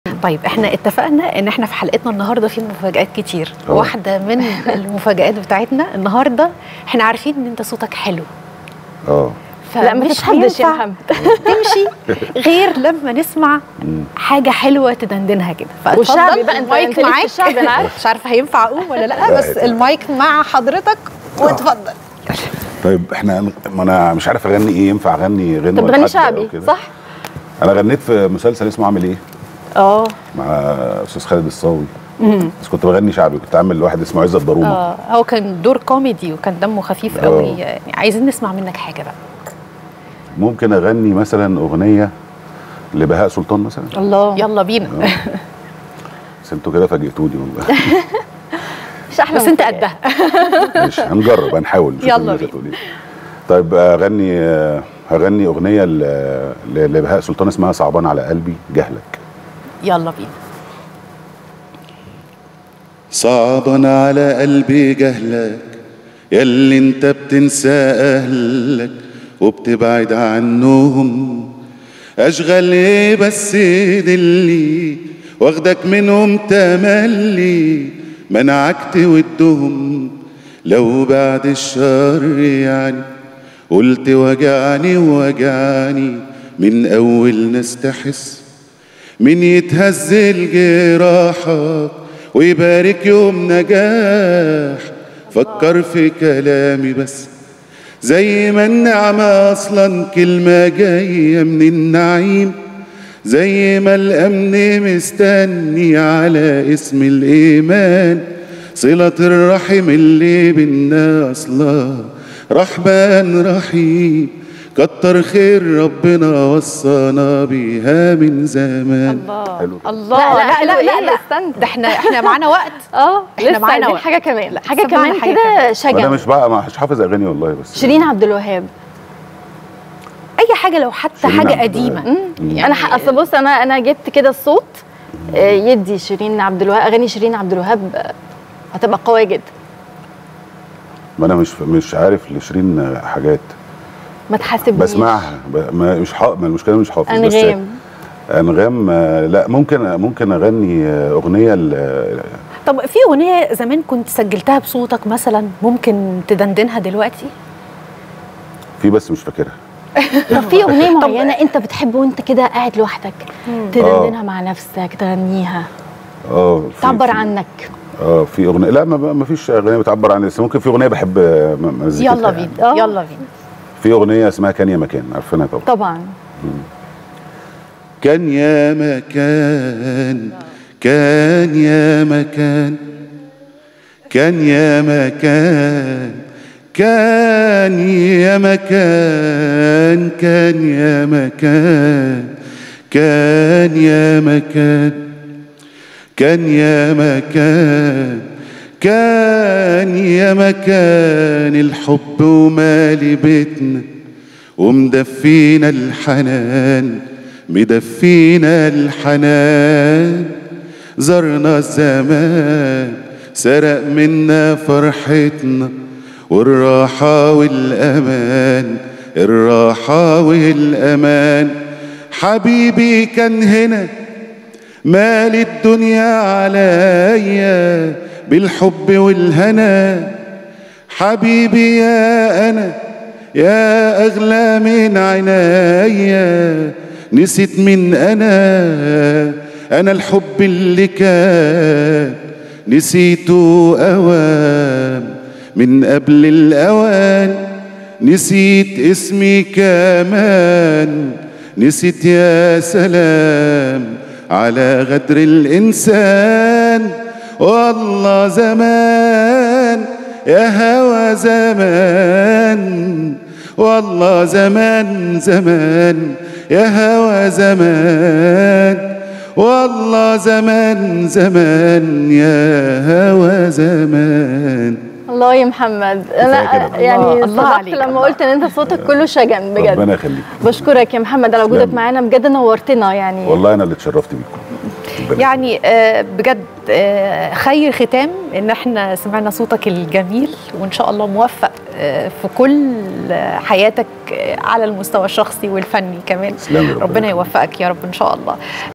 حصاد الدراما| بصوت كله شجن.. الفنان محمد عبد السيد يبدع في وصلة غنائية خاصة
16 مارس 2024